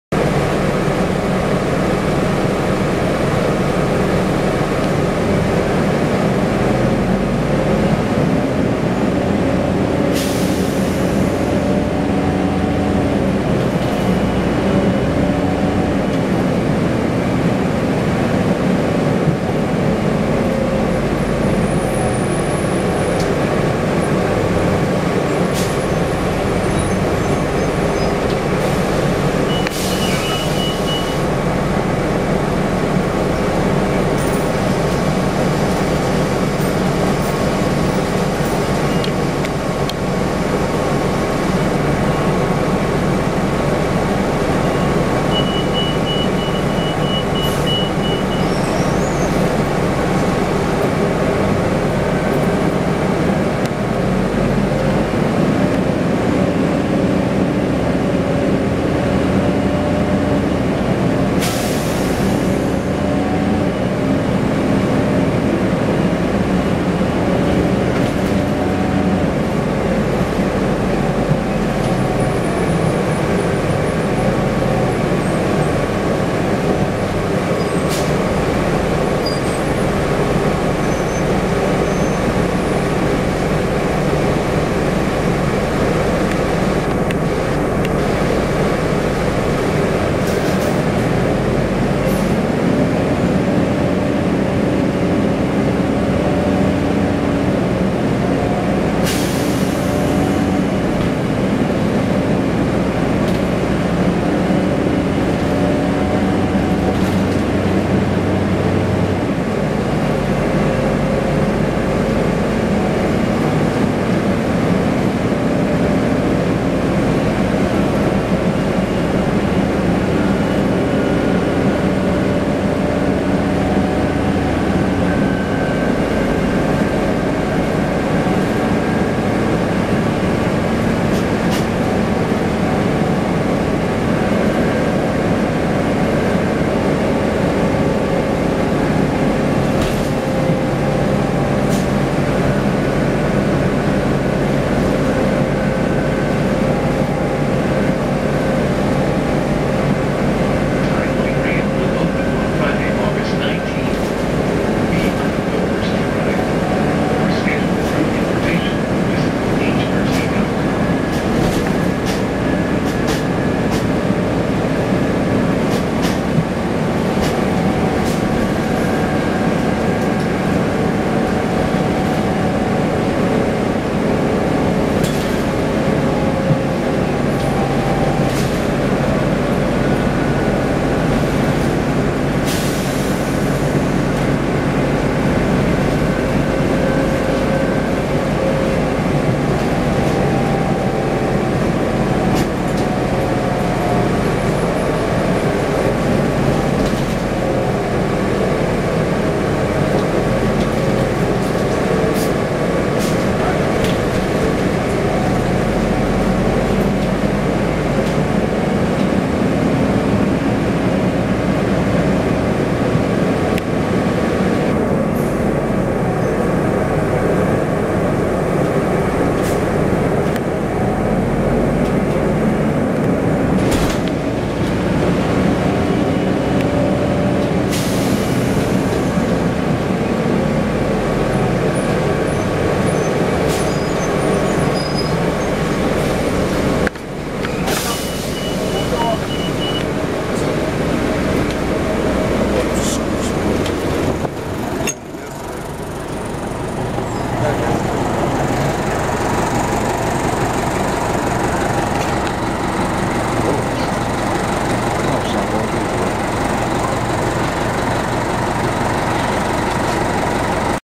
Hampton Roads Transit #513 (The Ride)
On the 106 (Warwick Blvd.) to Fort Eustis.

This is a 1995 Gillig Phantom with a Detroit Diesel S50 engine and Allison B400R transmission.